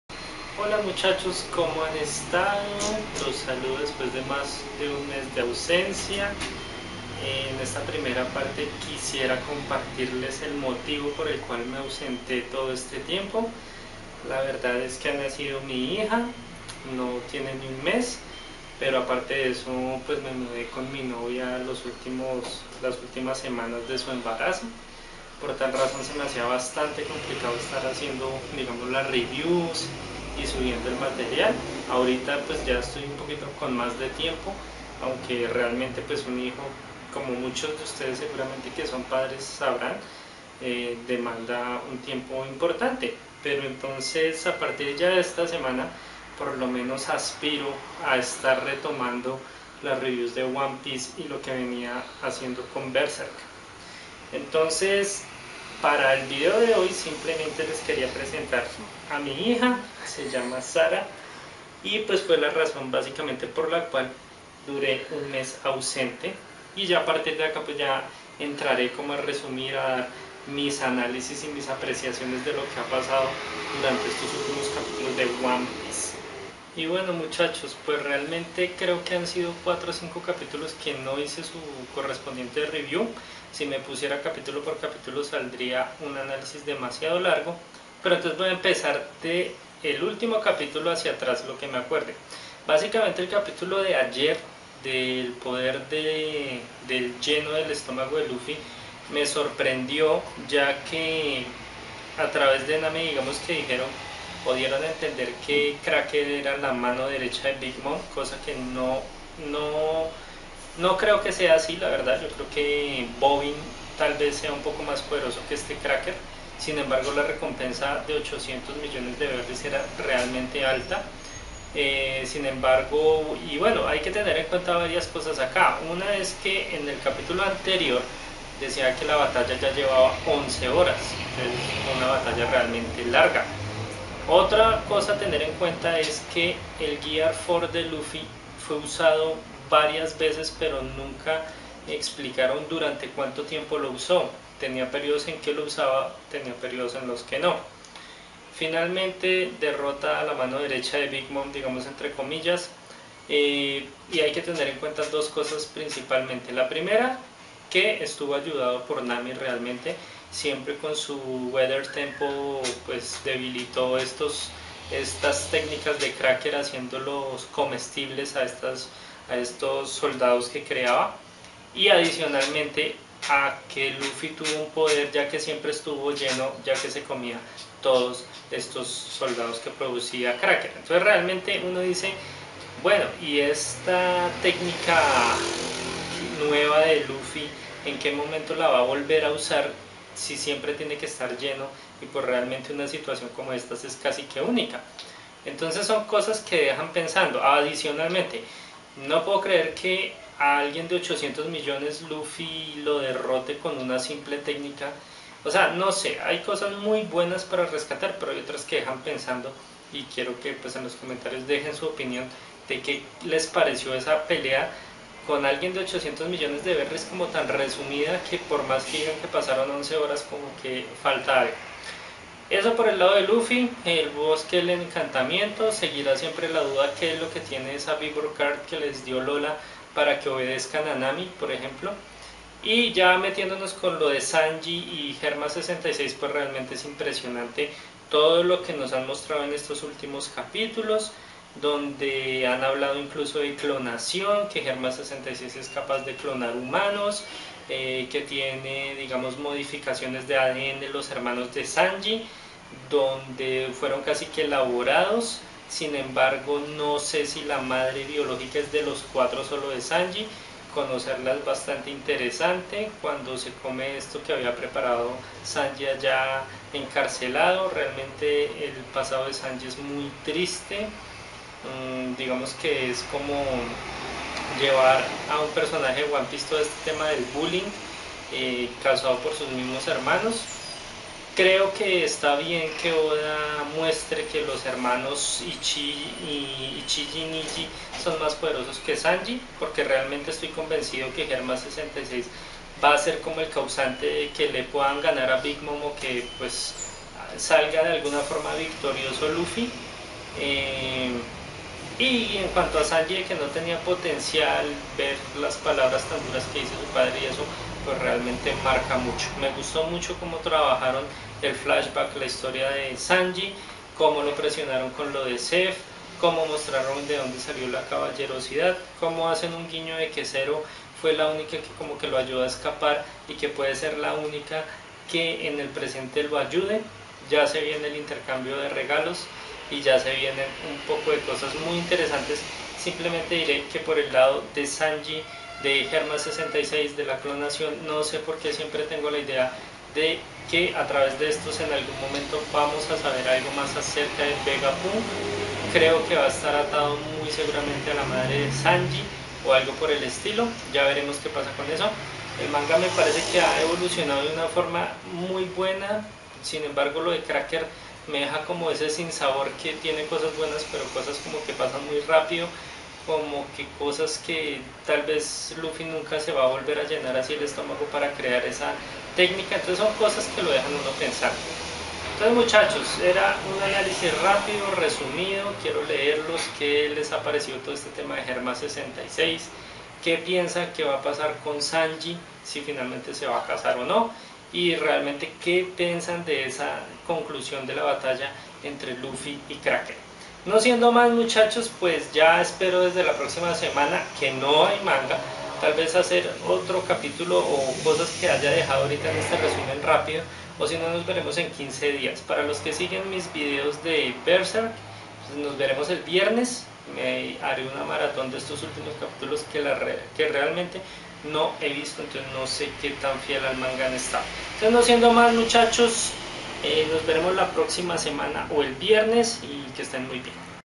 One piece 842 review y explicacion ausencia Shanks FP xD
Tras mes y medio estoy de vuelta y aca retomo un poco lo acontecido en one piece y les explico el porque de mi ausencia!

Gracias a quienes estuvieron pendientes y me escribieron, creanme que los ley pero para no generar falsas expectativas no respondi jeje